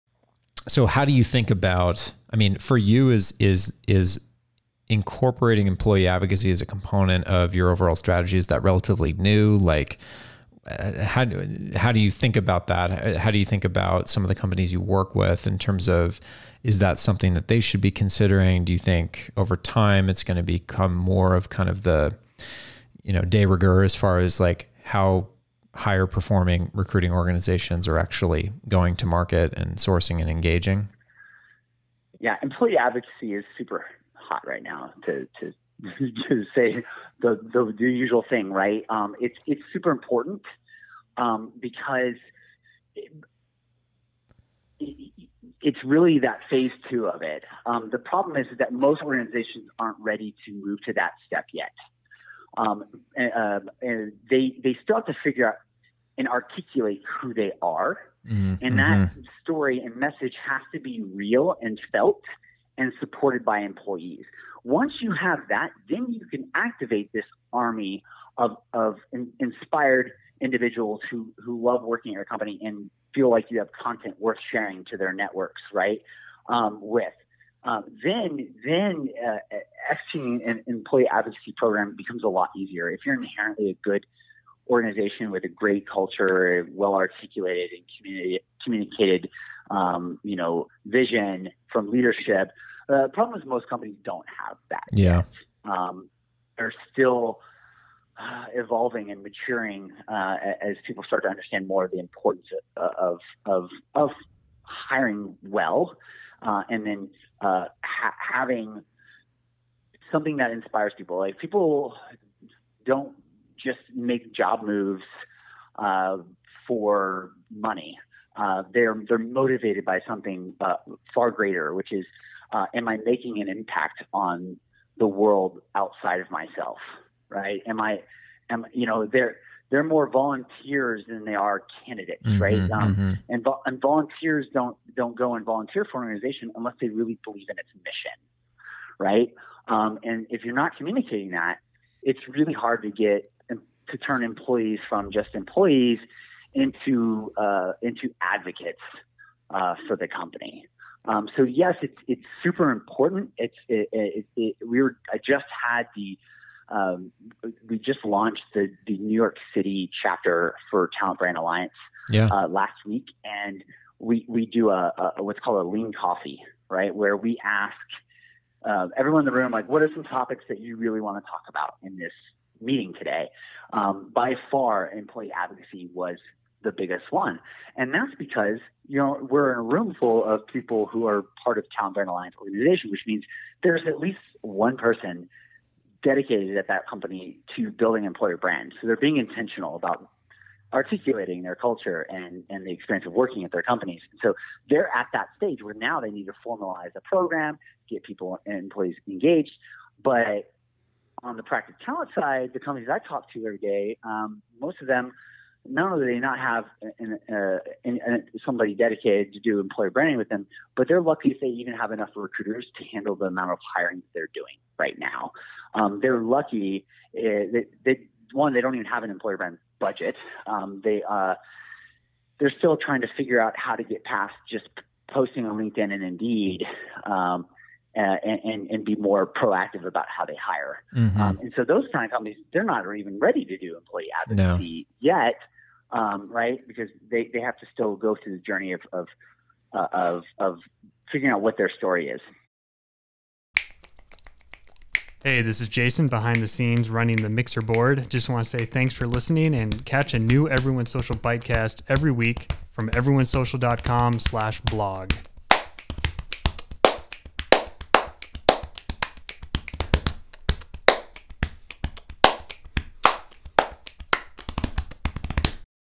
[Bitecast 11] Will Staney of Proactive Talent -- The Biggest Topic
This week's Bitecast episode features the founder of Proactive Talent, Will Staney who explains what actually motivates applicants -- why it is important for employees to communicate their company's mission, and why figuring out your brand's story lays the groundwork for a successful employee advocacy program.